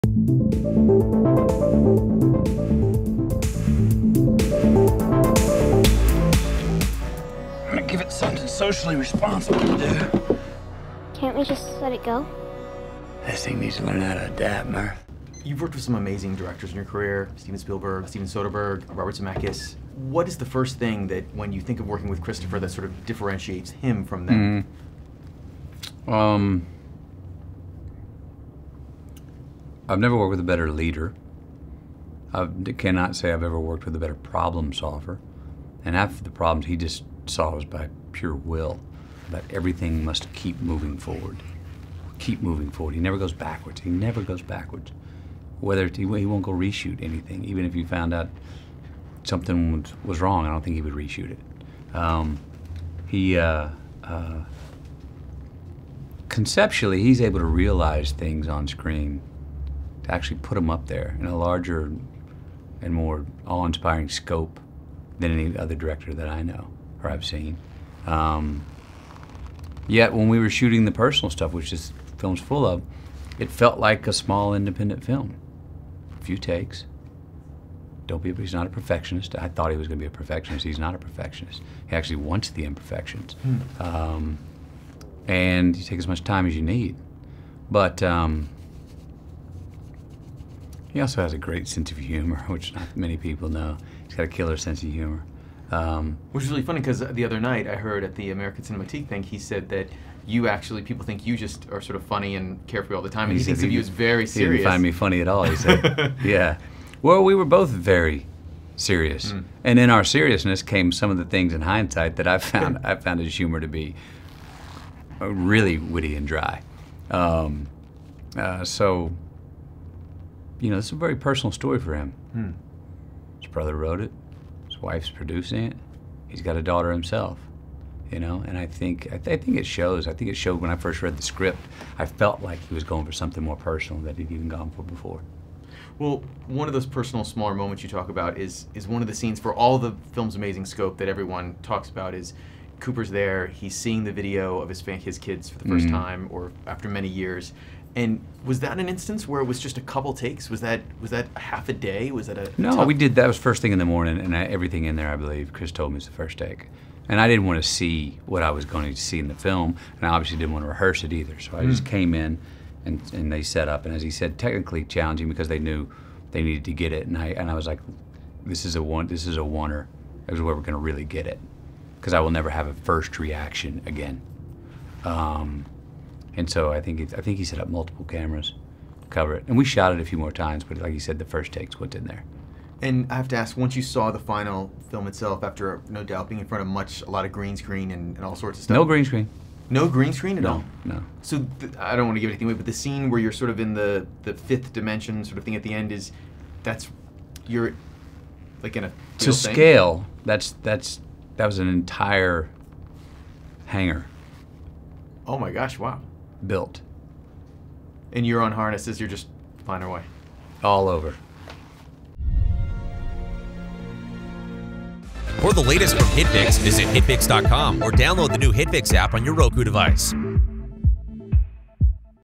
Matthew McConaughey on 'Interstellar' and Christopher Nolan as the best leader he's worked with
Matthew McConaughey on the process of making 'Interstellar'